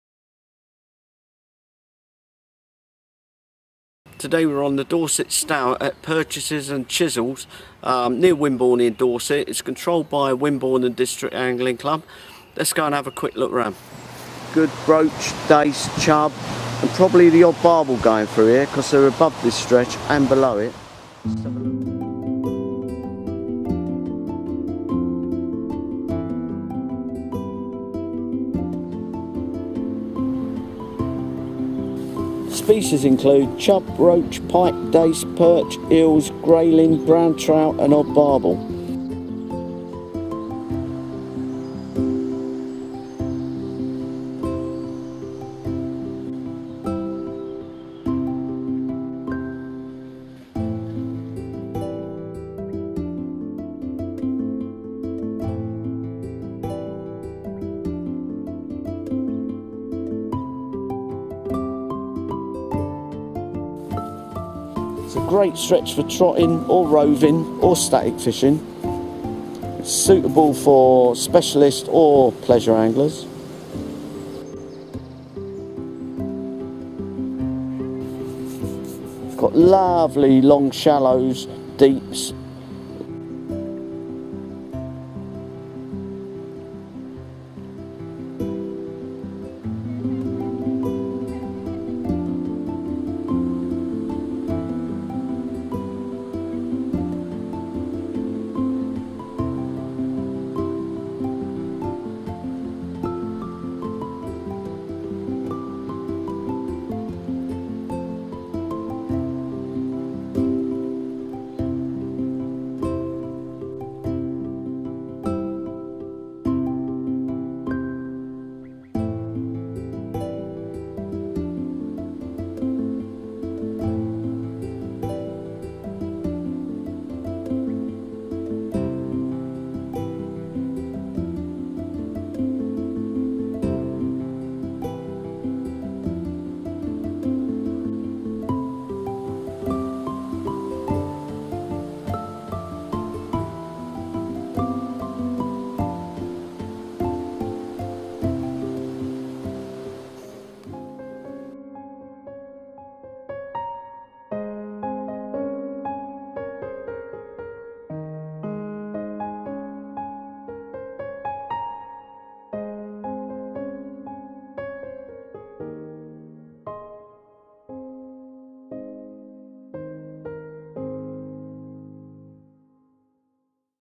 Ray Walton - DORSET STOUR, PURCHASE'S AND CHISSELL'S, CANFORD MAGNA, DORSET
Footage from summer 2013
Some of the info in this video may/will be out of date.
before travelling and intending to fish.
SAT NAV. BH21 3AD